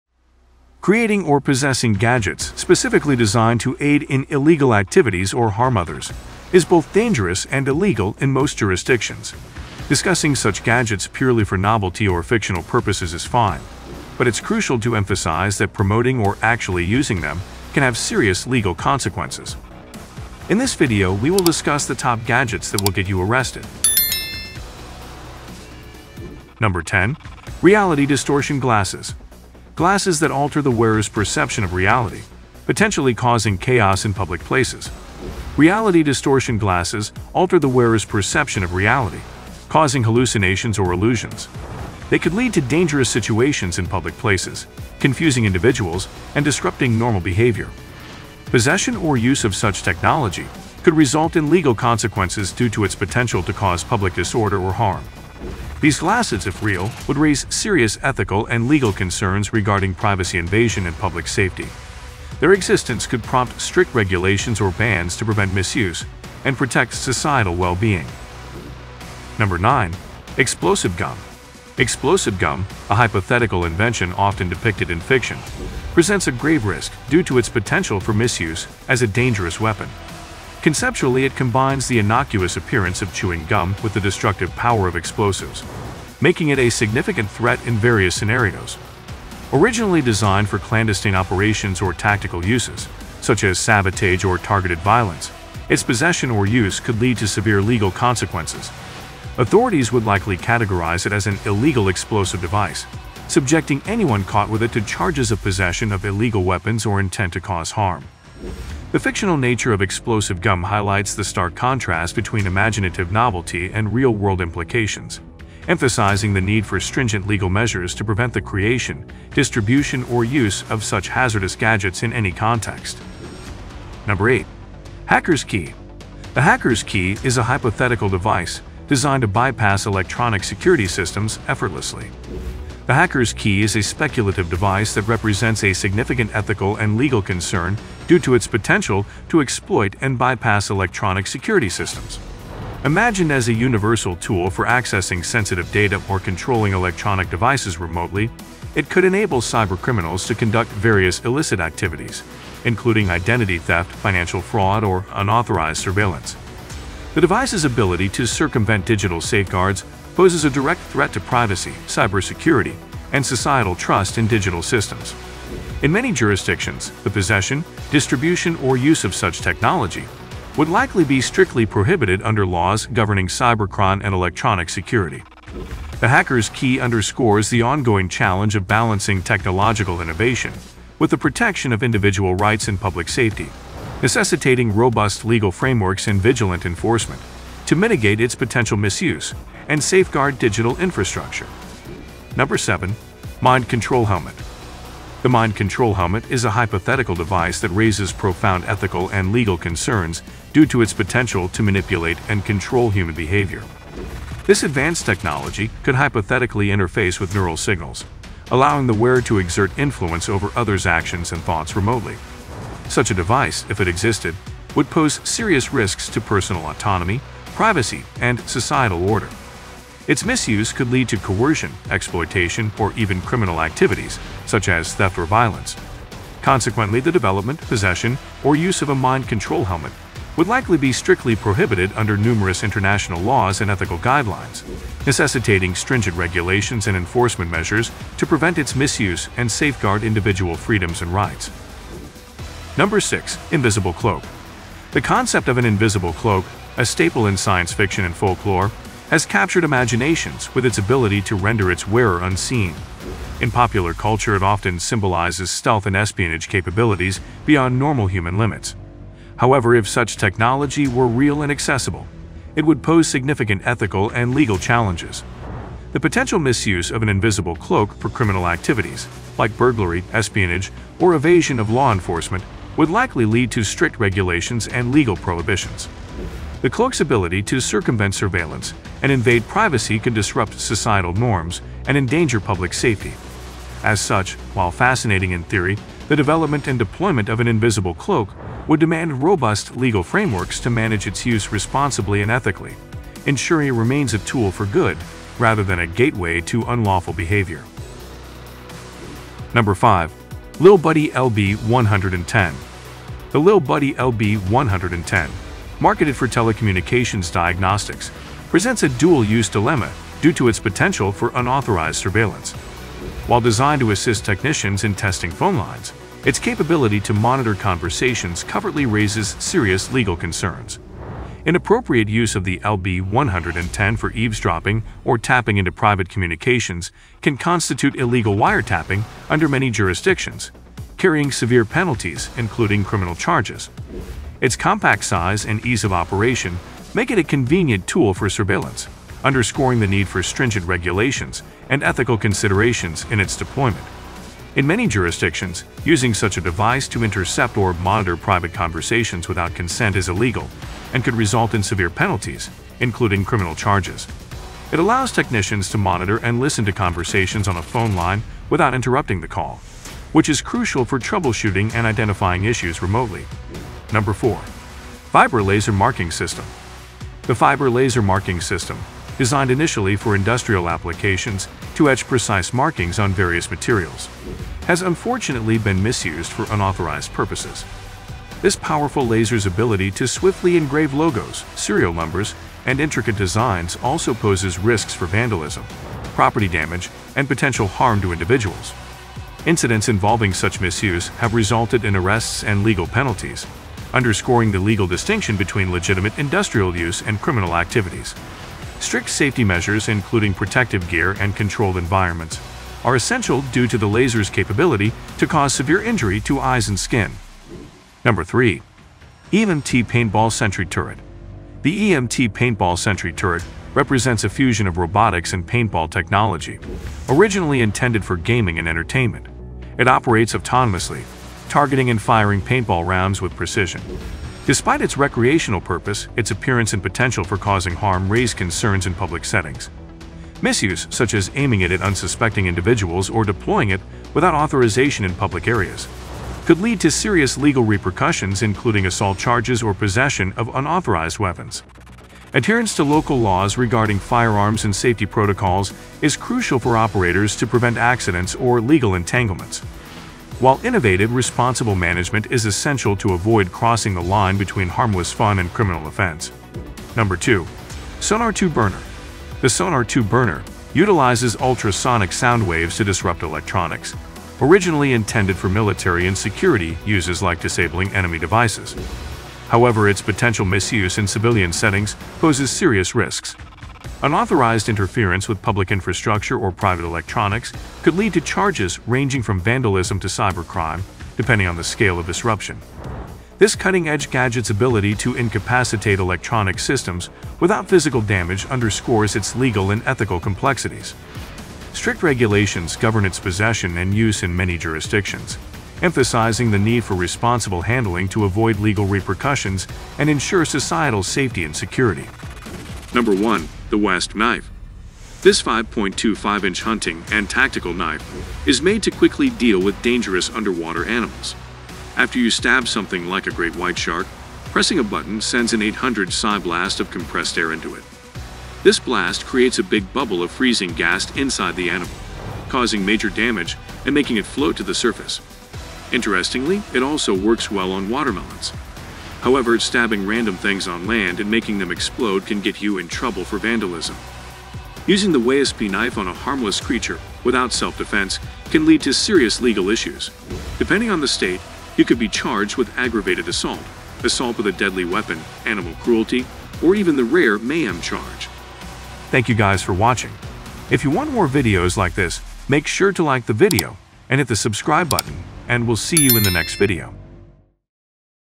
Crazy top gadgets that will get you arrested
craziest gadgets that, while innovative, could land you in serious trouble. From high-tech spy gear to devices that push the boundaries of legality, these gadgets are not only fascinating but also potentially dangerous. Discover what makes these items so controversial and why owning them could lead to an unexpected encounter with the law.